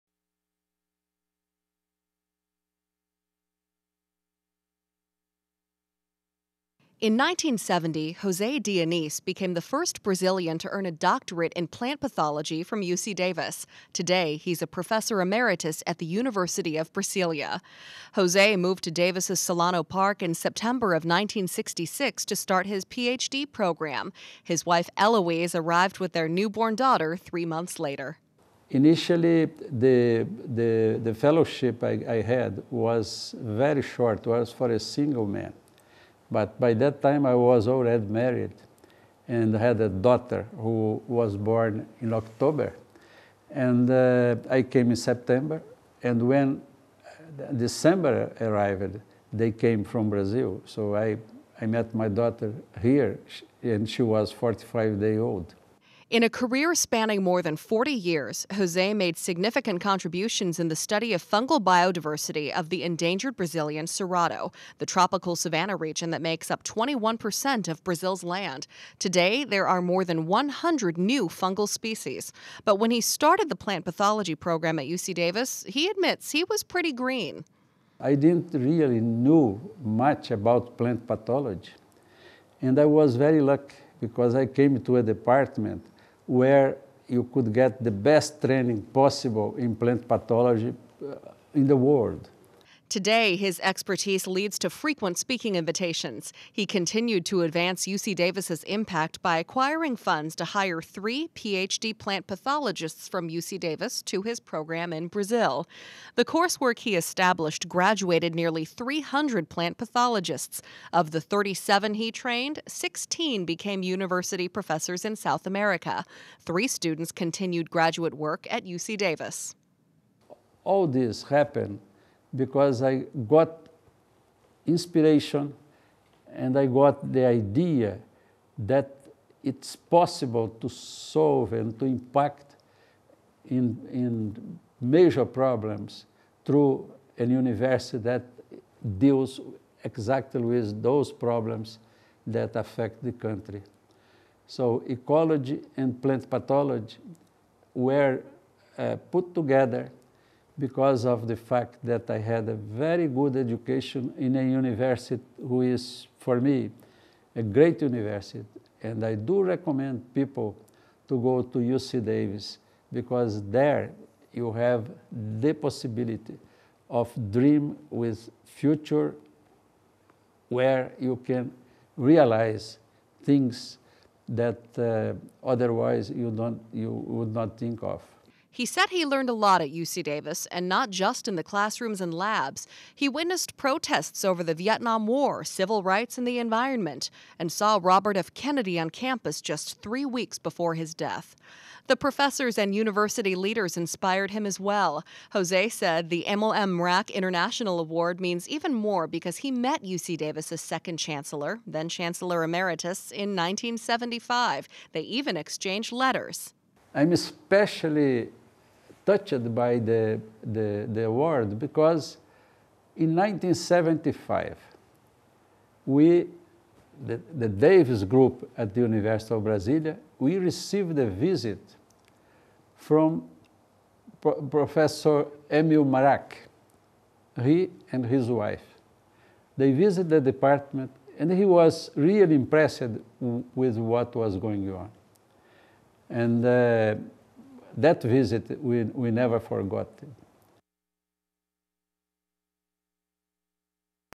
Jose Dianese, Mrak International Award
Jose Carmine Dianese M.S. '68, Ph.D. '71, is the 2013 winner of UC Davis' Emil M. Mrak International Award. He was honored on Feb. 1, 2013, at the Cal Aggie Alumni Association Award Gala in San Jose, Calif.